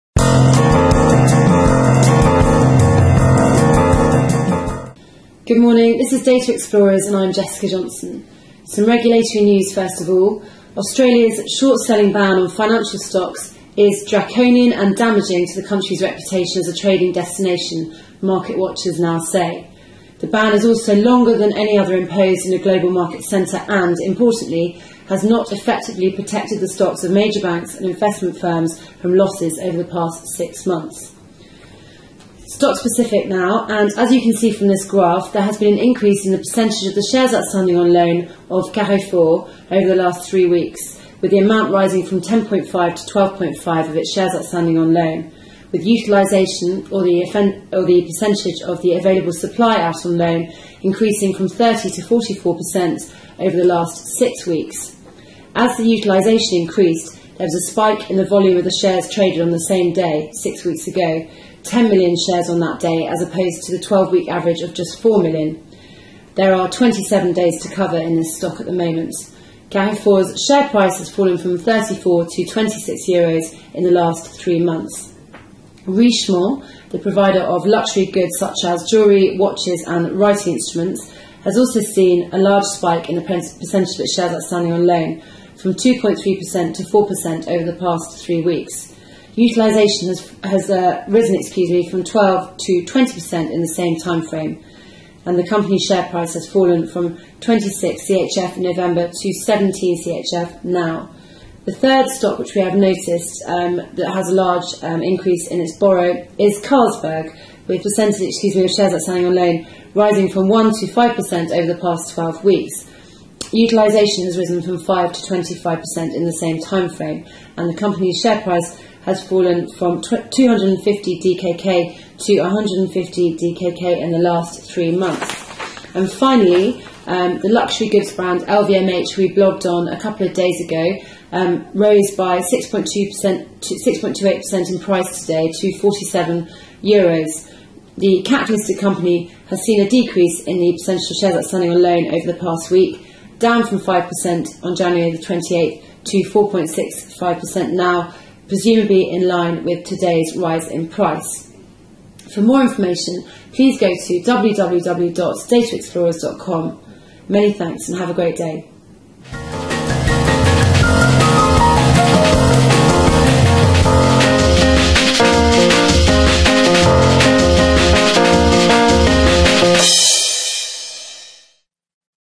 09 Feb 09 - Securities Lending Daily Brief - Data Explorers
Watch experts in Securities Lending and Short Selling present a brief update of the latest news and data for Securities Finance Professionals and Investment Managers.